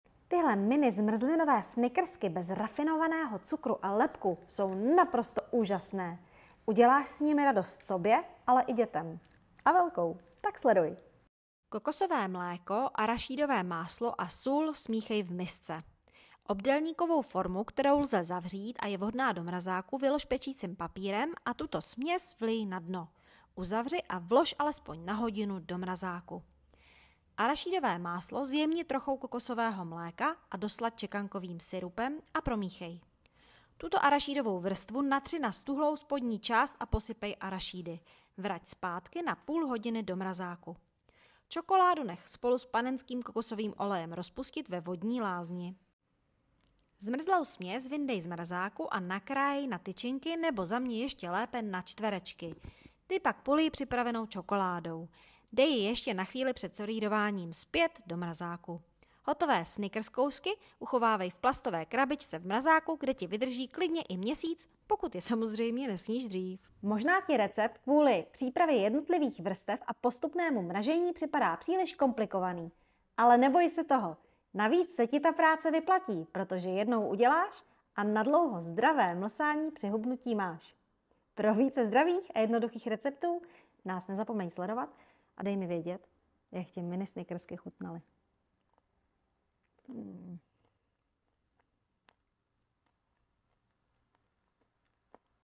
Recept na božské zmrzlinové mini snickersky
Tyto zmrzlinové mini snickersky bez rafinovaného cukru a lepku jsou naprosto úžasné! Sobě i dětem s nimi uděláš radost a velkou - tak sleduj!

Týna 🍫
PS: celý recept s postupem a surovinami najdeš i na blogu: